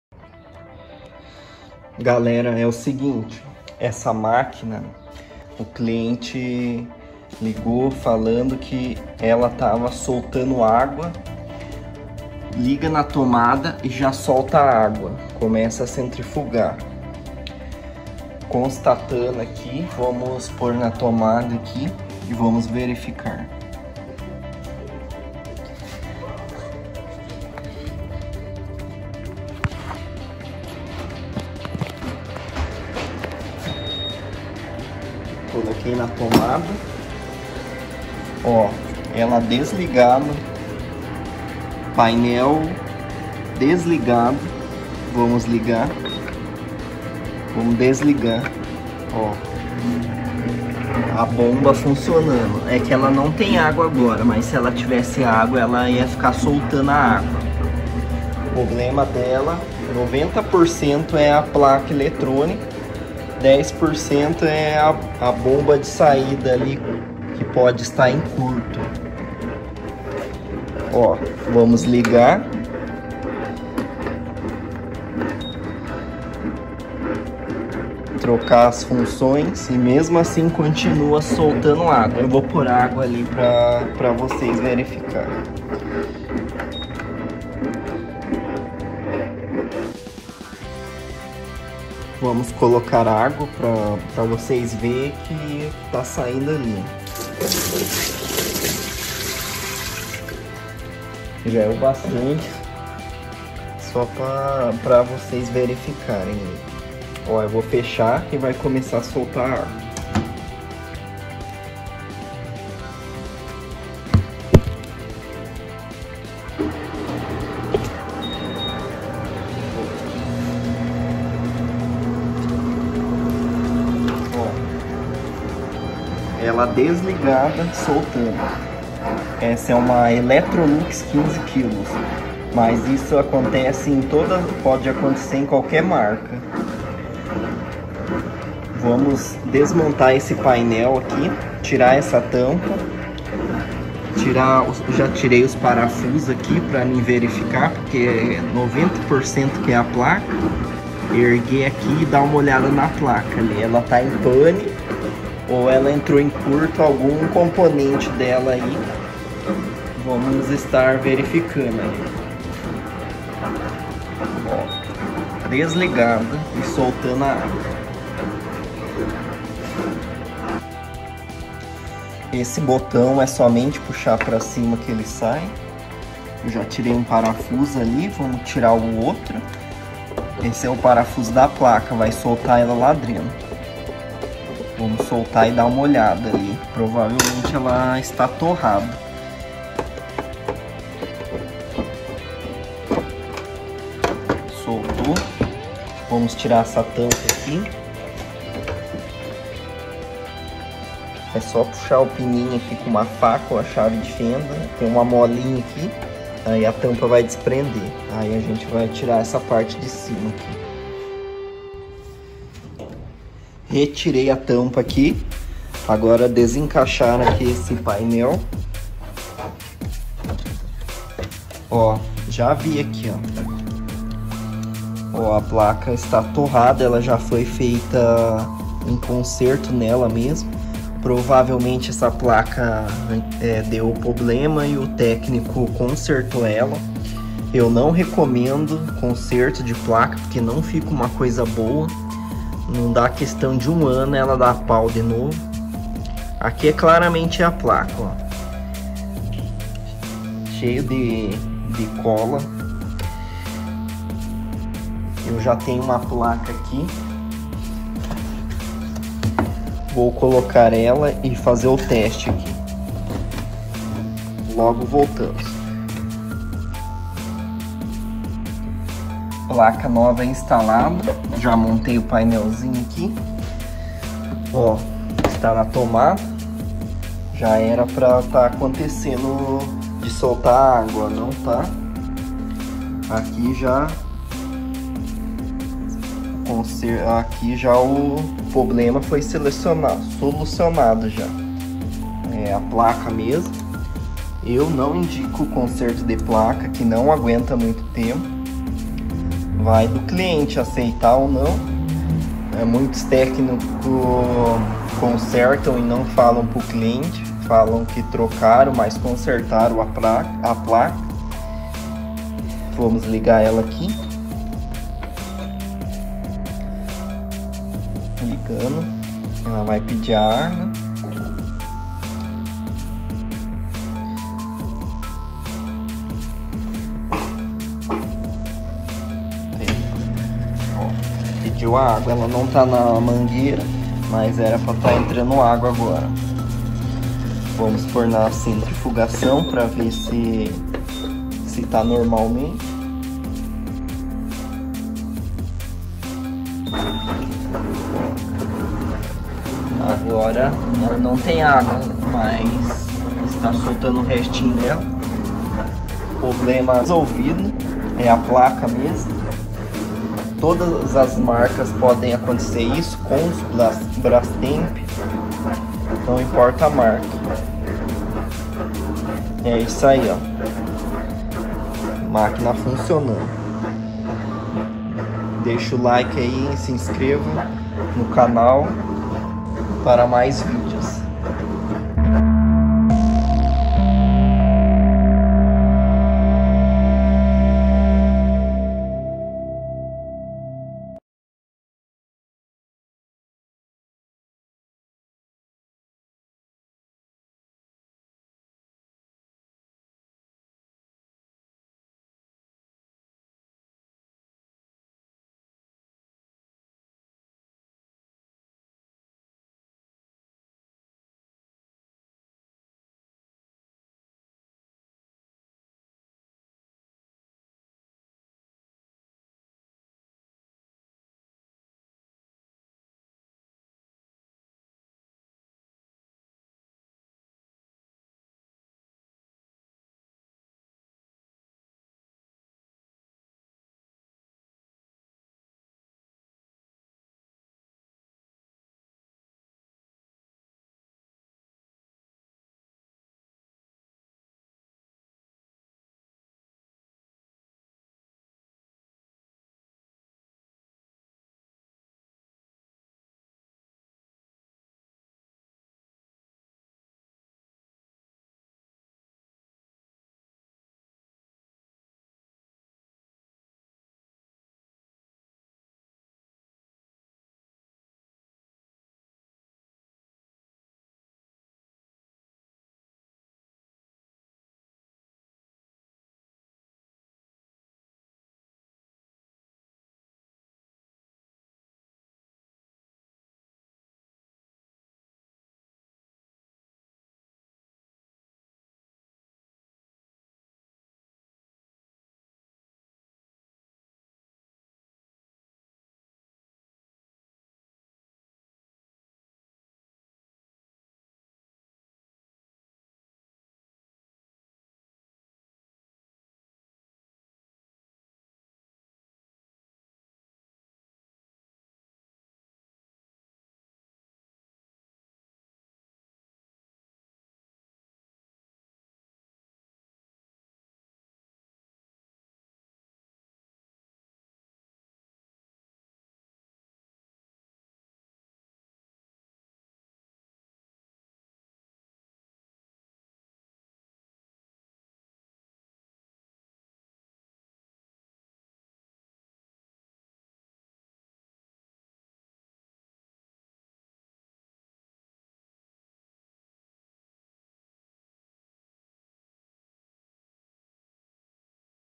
Entra agua e já sai ao mesmo tempo pela Mangueira
Maquina de lavar Electrolux 15kg
funcionando desligada .
Placa e eletrobomba em curto